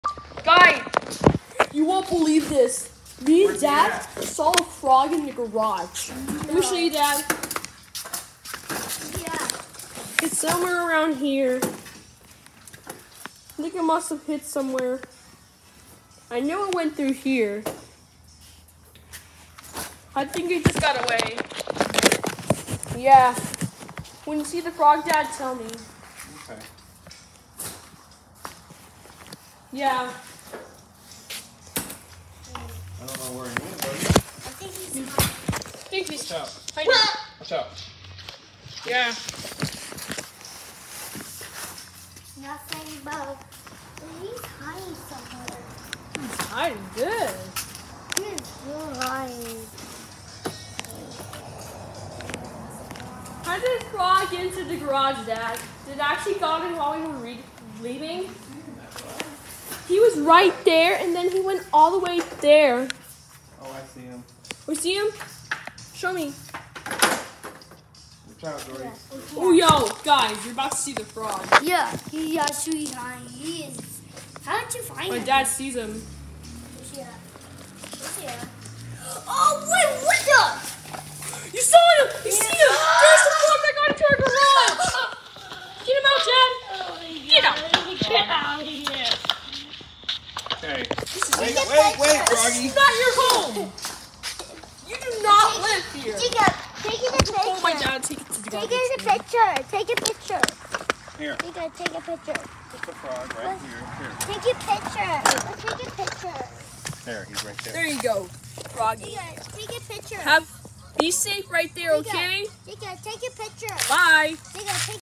OMG there was a frog 🐸 in my Gurage but we fully got it out
It all started when I got home from dropping off my sister from soccer practice And when we open the garage I saw the frog￼ That hided In our bicycles and our scooters￼￼ and my dad use a broom to get the frog home. And then when we scooped it out with the broom before you pull in by the tree where my dad Silverado was￼ So he can have a safe travel￼. like and subscribe if you had this experience￼￼ before like and subscribe if you love frogs 🐸 .￼